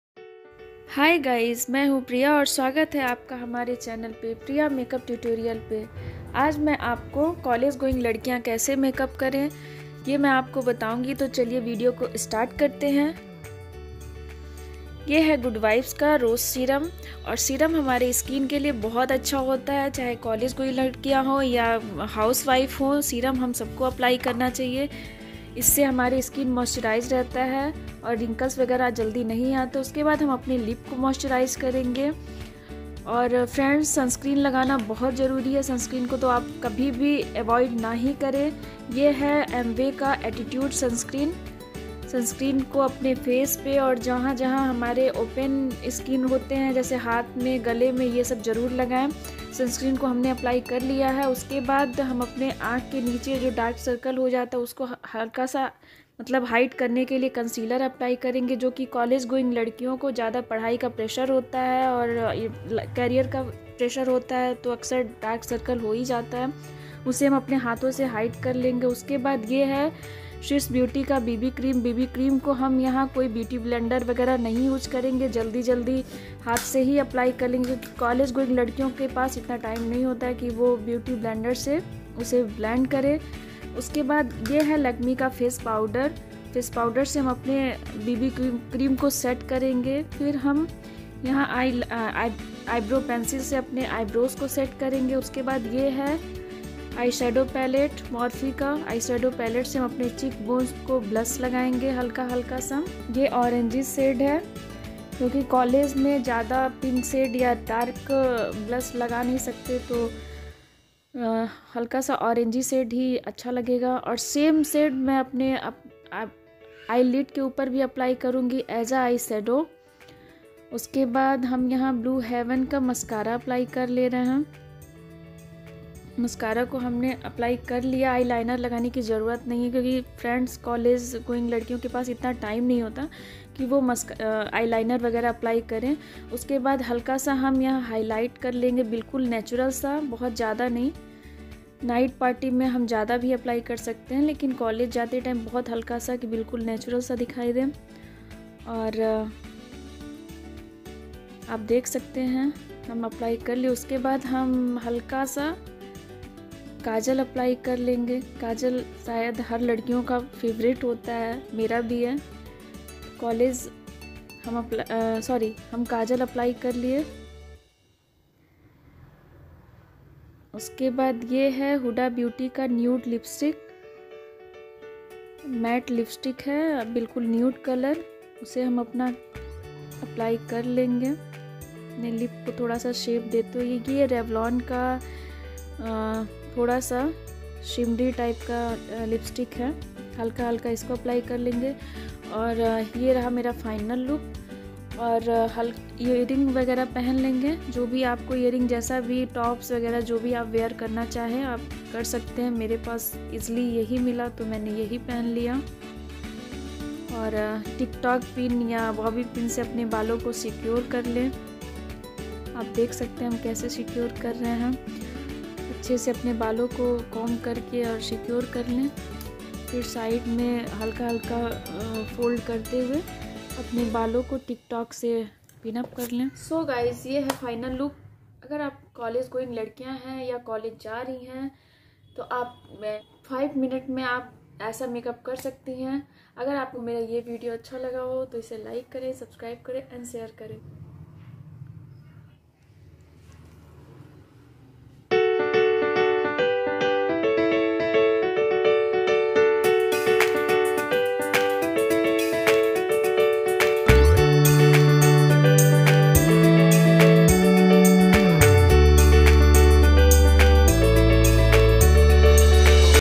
college going girls aise kare Makeup # priya Makeup tutoriyal#
priya Makeup tutoriyal#

good Vibes Rose Hip Serum
Amway  attitude sunscreen
Kiss Beauty concealer
SWISS Beauty bb cream
morphe eyeshadow
a.d.s eyebrow Pencil
huda Beauty Matte nude Color Lipstick
revlon Lipstick